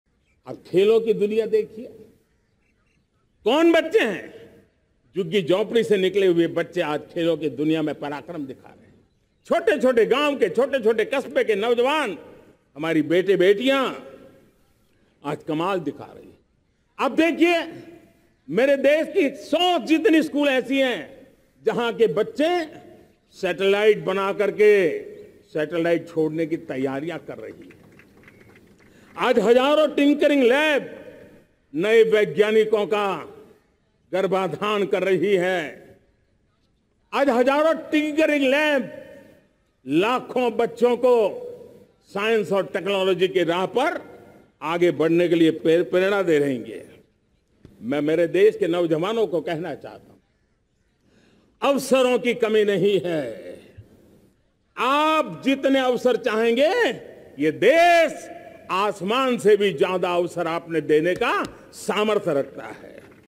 PM Narendra Modi addresses the Nation on Independence day.
आज झुग्गी-झोपड़ी से निकले बच्चे दुनिया में पराक्रम दिखा रहे हैं। छोटे-छोट गांव, कस्बे के नौजवान, हमारे बेटे-बेटियां आज कमाल दिखा रहे हैं। - Narendra Modi